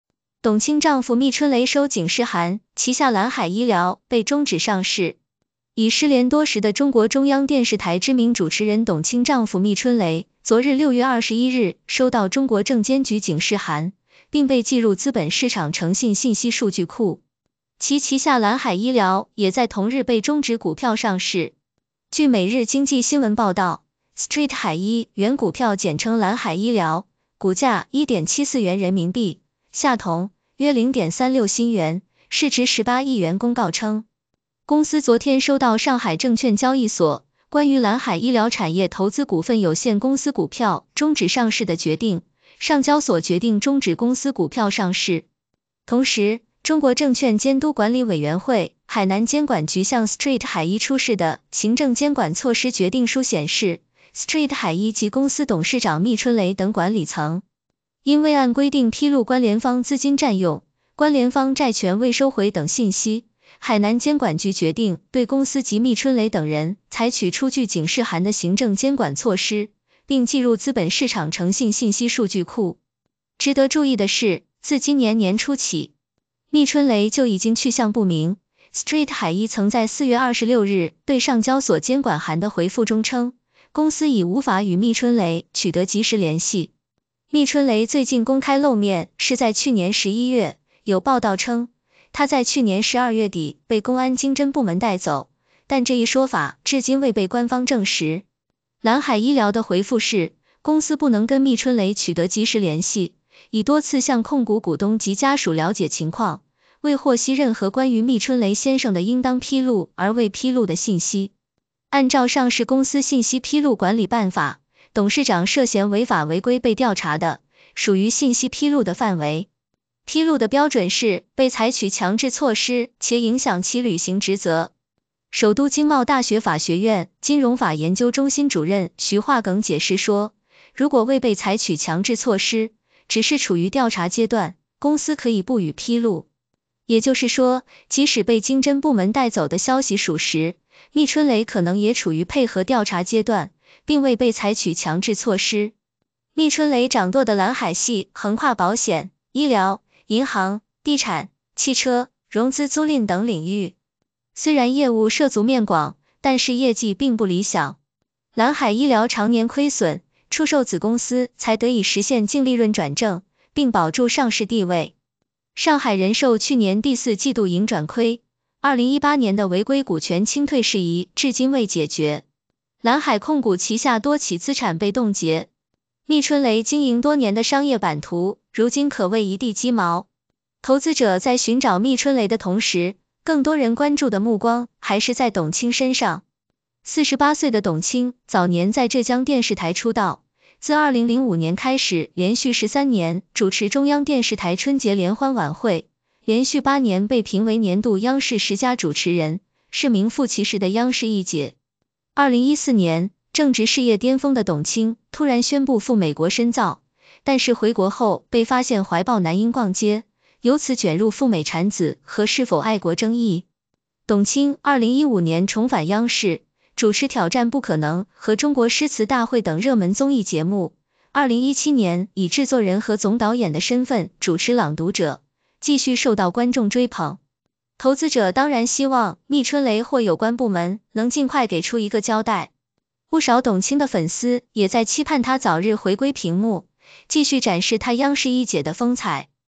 央视一姐董卿丈夫密春雷收警示函 旗下览海医疗被终止上市
已失联多时的中国中央电视台知名主持人董卿丈夫密春雷昨日（6月21日）收到中国证监局警示函，并被记入资本市场诚信信息数据库，其旗下览海医疗也在同日被终止股票上市。
据每日经济新闻报道，*ST海医（原股票简称览海医疗，股价1.74元人民币，下同，约0.36新元，市值18亿元）公告称，公司昨天收到上海证券交易所《关于览海医疗产业投资股份有限公司股票终止上市的决定》，上交所决定终止公司股票上市。